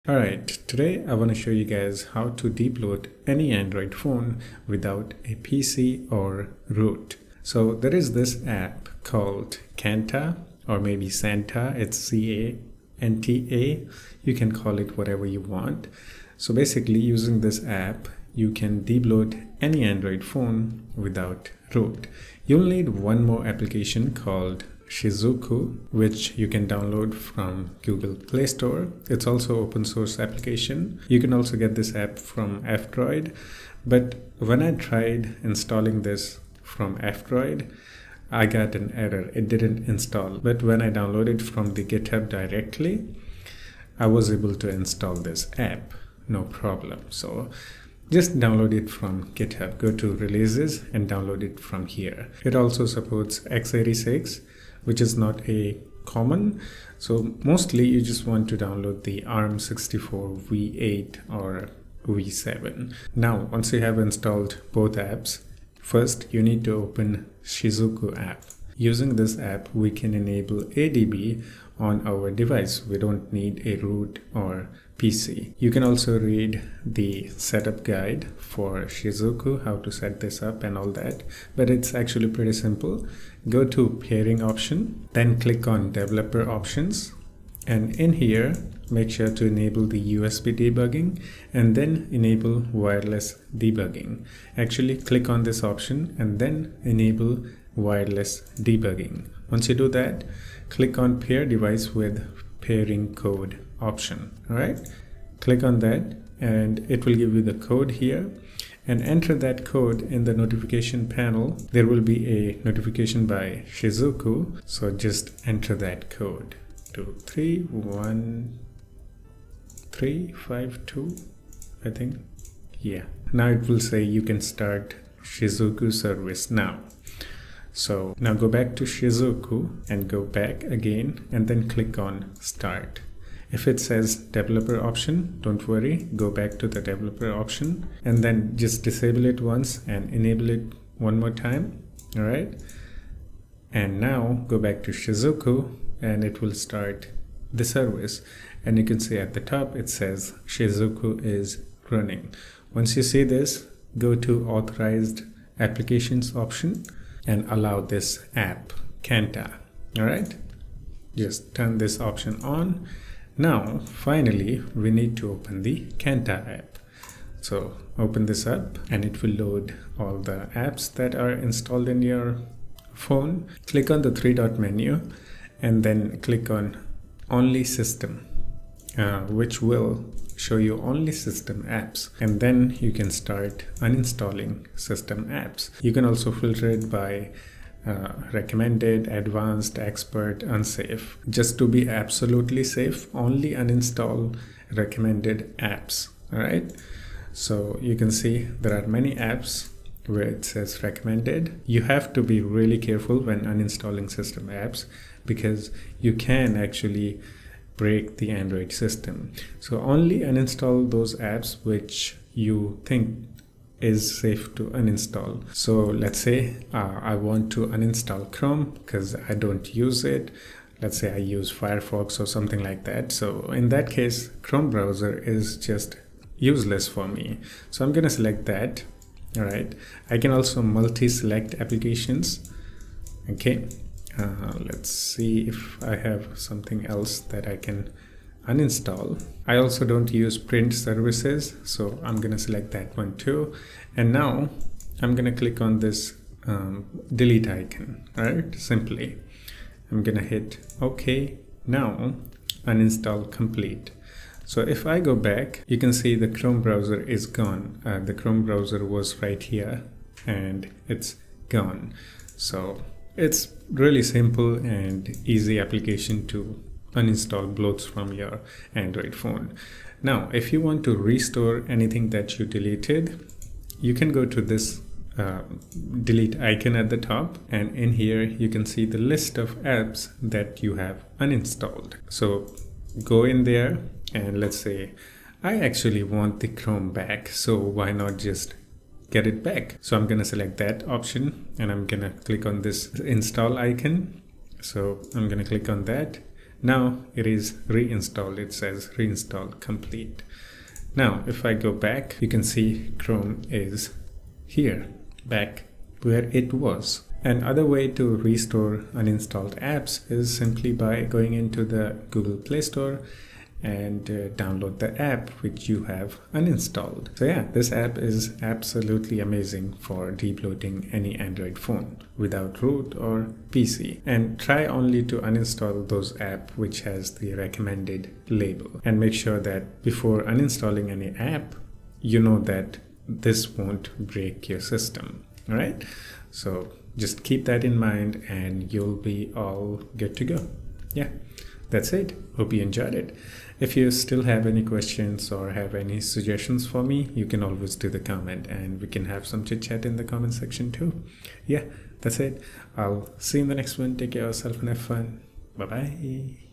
Debloating Android without PC or Root
How to debloat any Android phone without a PC or Root easily 👍